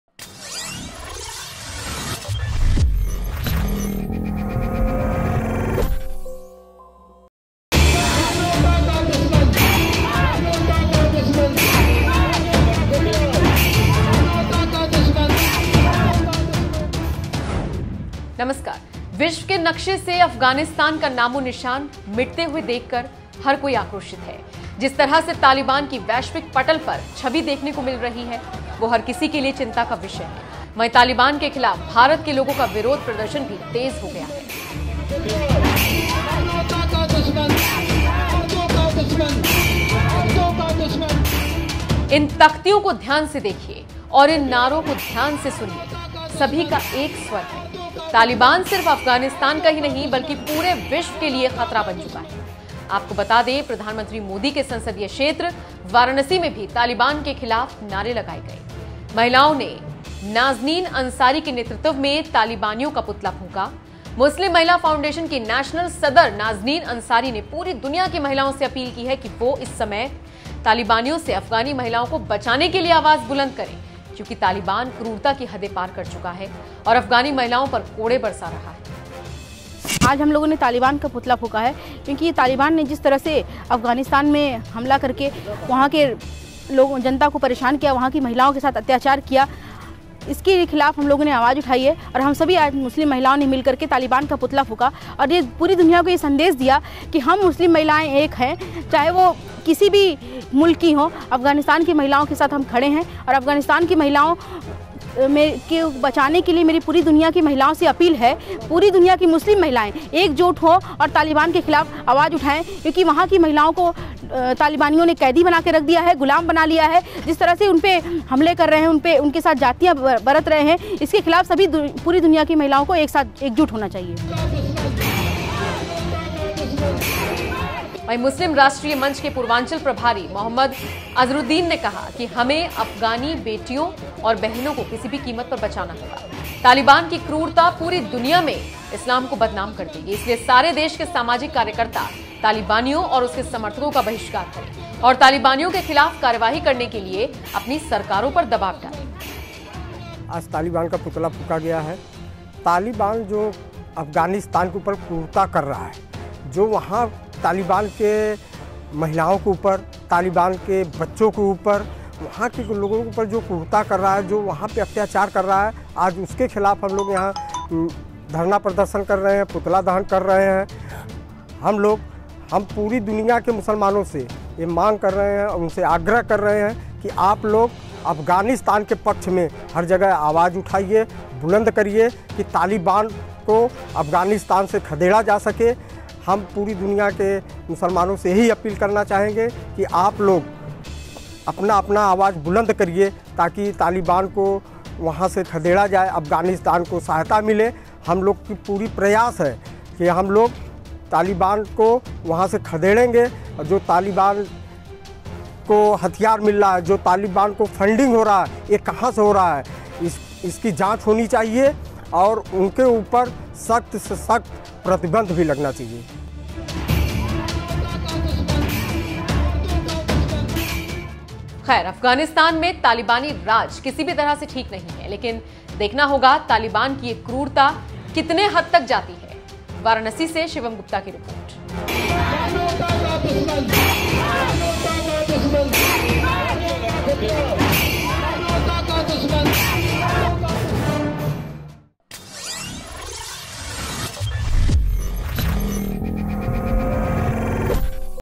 India के मुसलमानों ने तालिबानियों का देखिये कैसे जनाजा निकाला ?
अफगानिस्तान में तालिबान की बढ़ती क्रूरता को देखकर भारत में भी विरोध के स्वर उठने लगे हैं.भारतीय मुसलमानों ने तालिबान के खिलाफ विरोध प्रदर्शन किया और पुतला फूंका